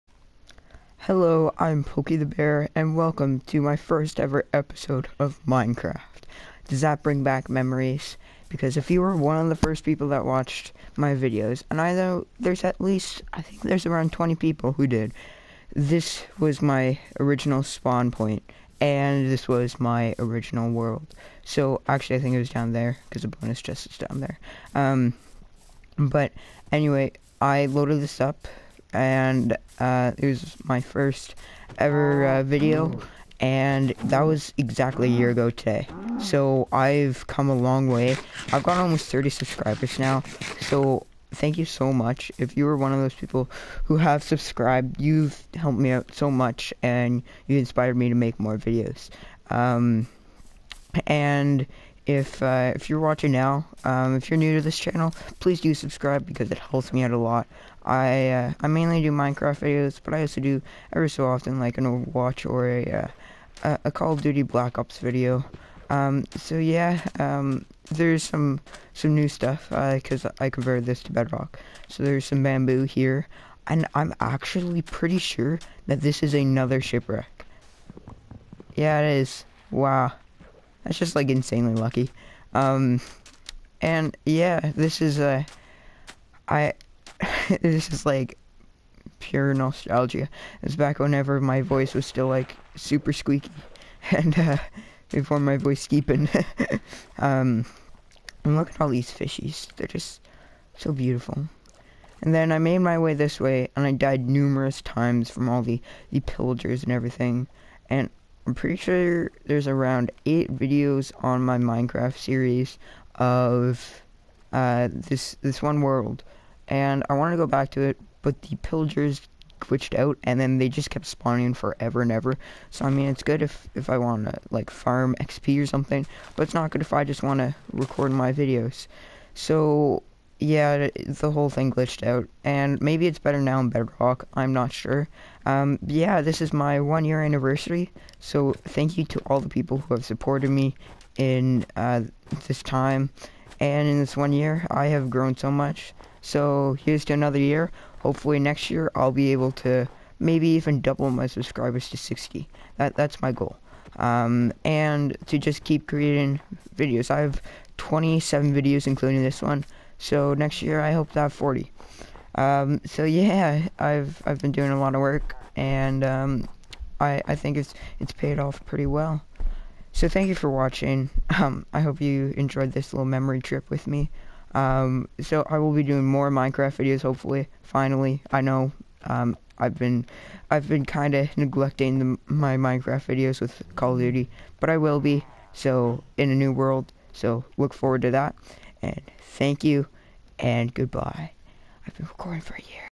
My 1st Anniversary!
1 year ago, on this day, I made my first video. I was still a squeaky little boy, me and this community has grown so much in the last year. Thank you for all your support and keep being awesome! I will be uploading once a week from now on.